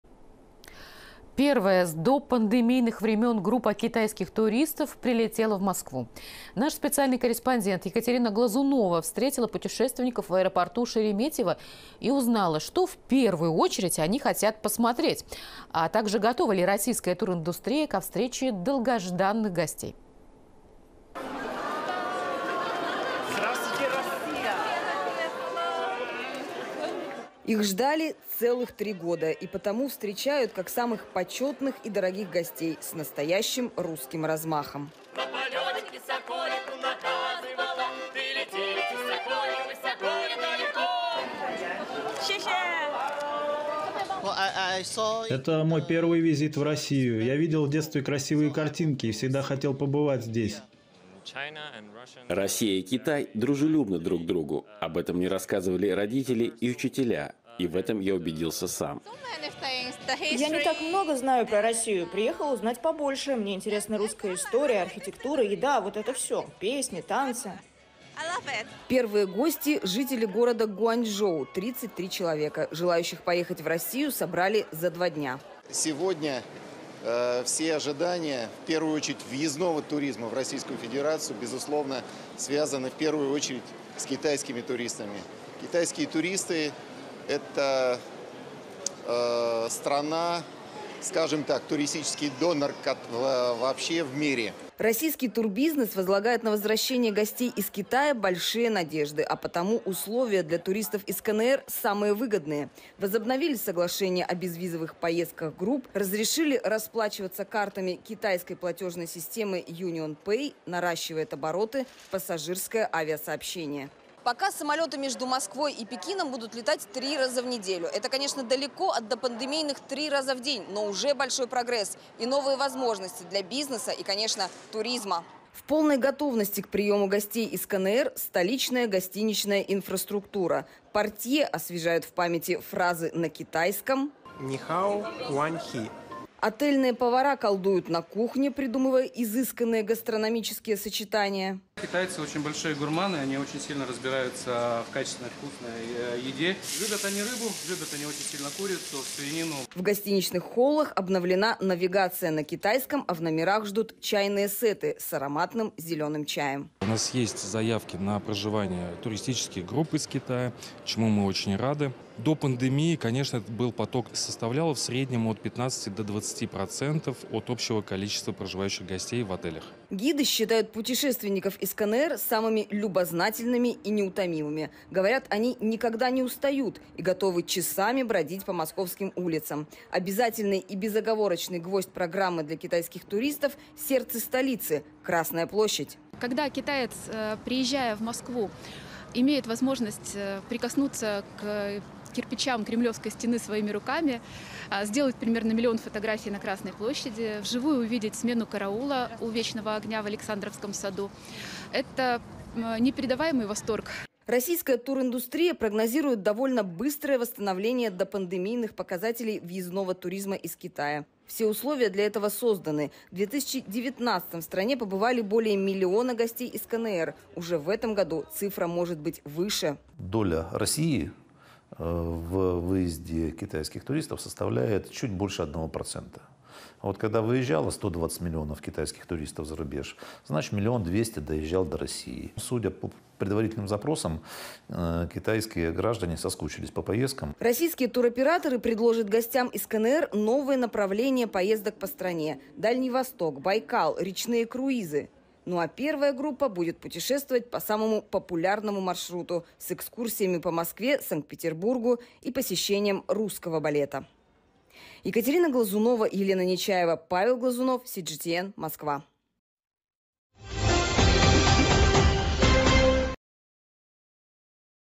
Первая организованная группа китайских туристов приехала в Москву
Первая с допандемийных времен группа китайских туристов прилетела в Москву. Что в первую очередь хотят посмотреть китайцы, а также готова ли российская туриндустрия к встрече долгожданных гостей - узнаете из нашего видео.